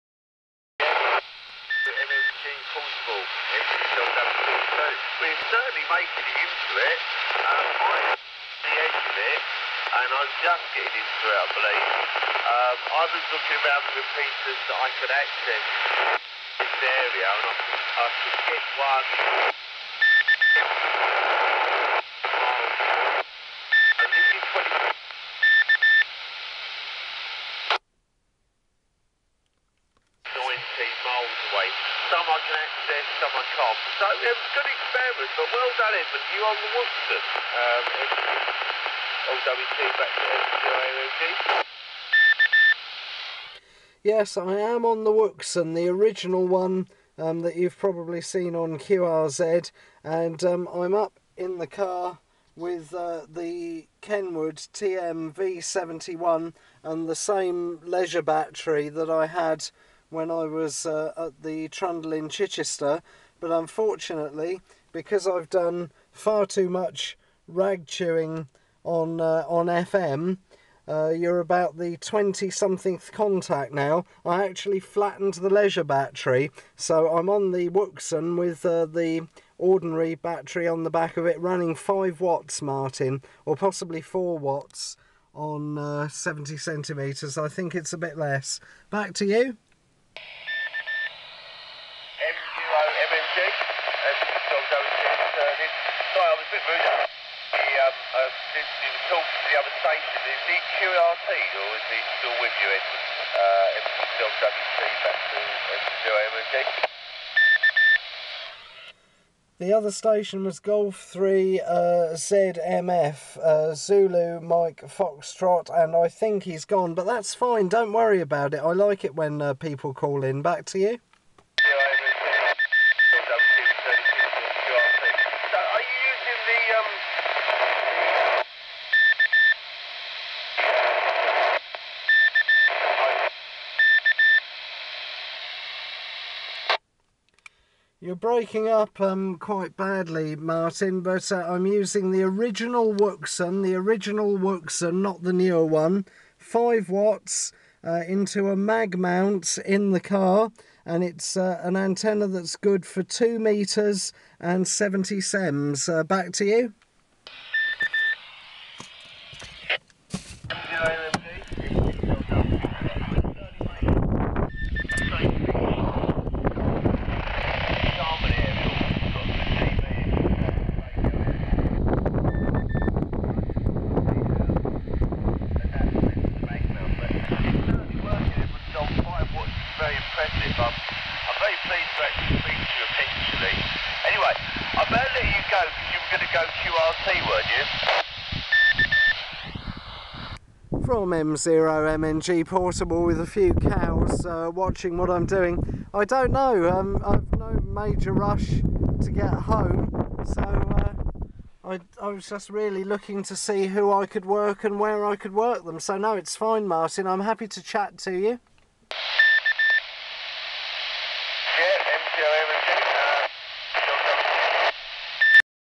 M6LWT in QRP QSO with MOMNG/P via GB3NS 27.05.2013
M6LWT in conversation with M0MNG/P and QRP.  Both stations were right on different edges of the repeater's coverage area and both were running low power; the Wouxon KG-UVD1P outputs 4 Watts on 430MHz.

M0MNG was on top of Chantry Hill, near Sullington and Storrington.  Other stations I spoke with informed me that Chantry Hill car park is in WAB Square TQ01 and is 180 meters above sea level.

This was just one of 24 QSOs in four hours, of which 20 were on 2 meters FM with stations in West Sussex, East Sussex, Hampshire, the Isle of Wight, and Devon.